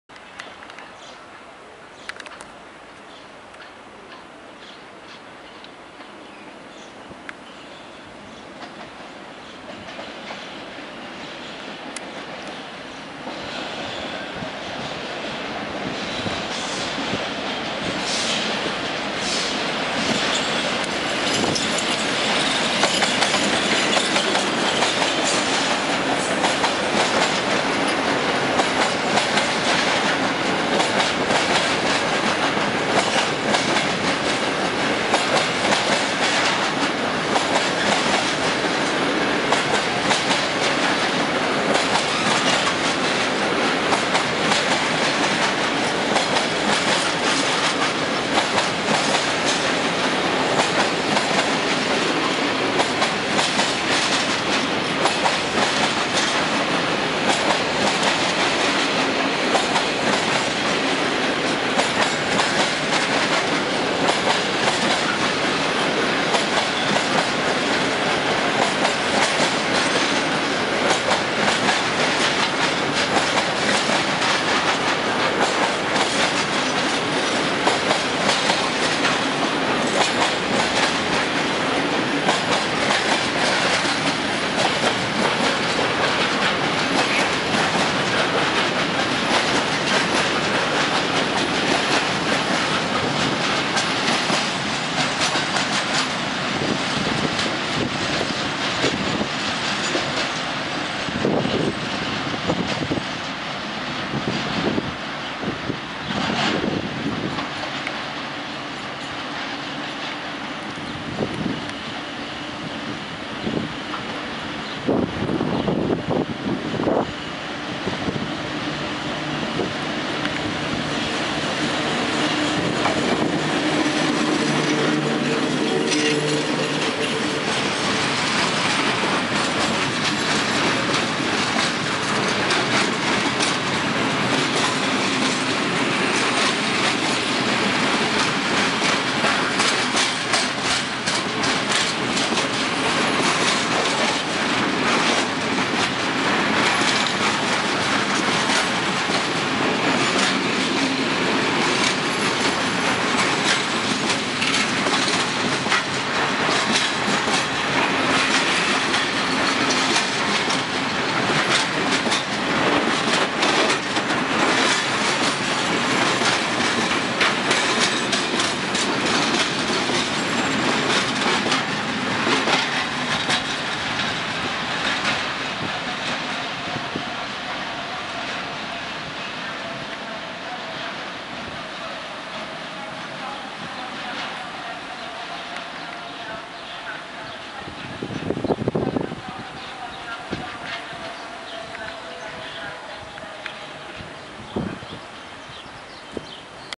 Mijanka ET22-320 oraz ET22-140 [Częstochowa Osobowa]
Mijanka ET22-320 oraz ET22-140 widziana z peronu nr 1 stacji Częstochowa Osobowa. Życzę miłego oglądania ! Nagrano: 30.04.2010r. o godz. 13:37.